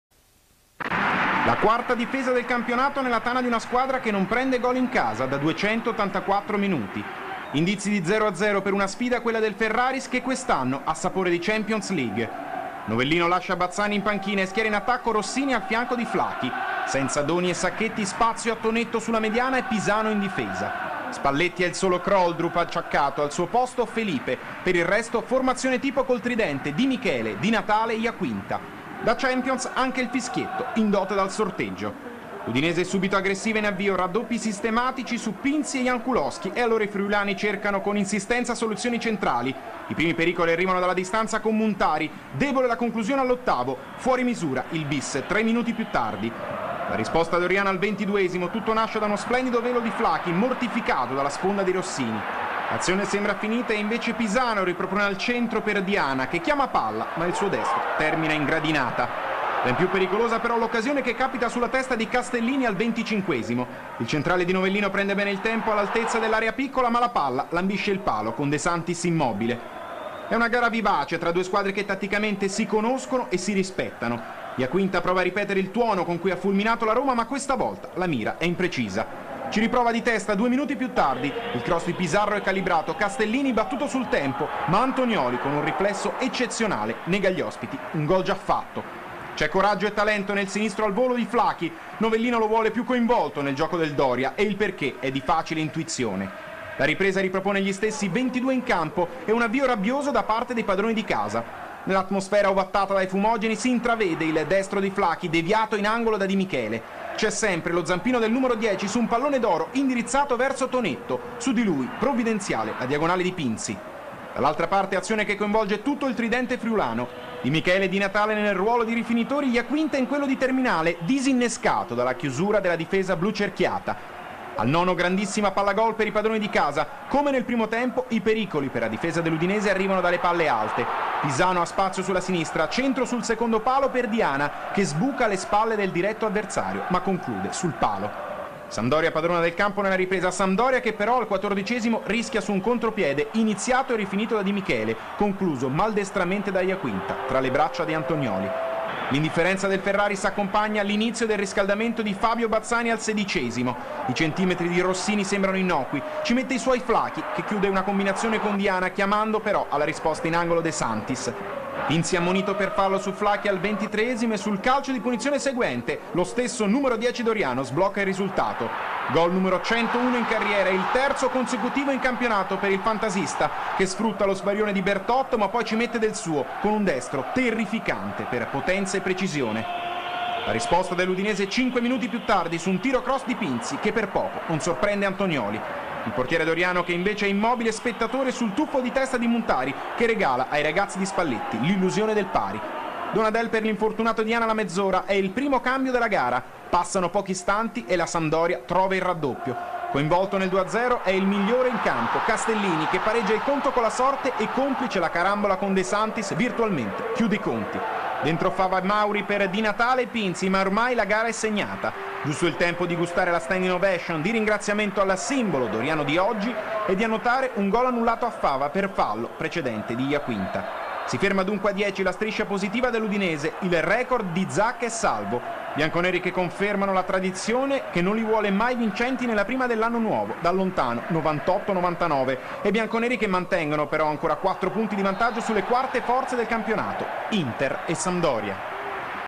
Sampdoria 2-0 Udinese - Campionato 2004/05
17° Giornata del Campionato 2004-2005

Goals :
68' F.Flachi (SAMPDORIA)
75' M.Castellini (SAMPDORIA)